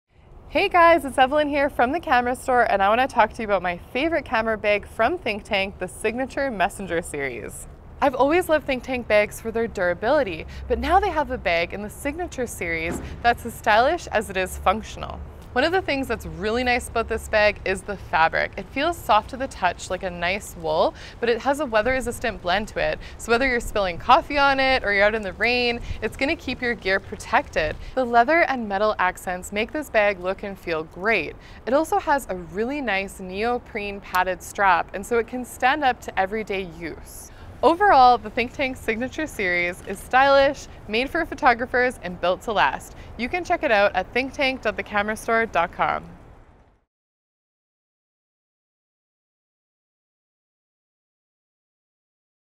ThinkTank Signature Series
Find out why Evelyn is in love with her ThinkTank Signature Series Messenger Bag.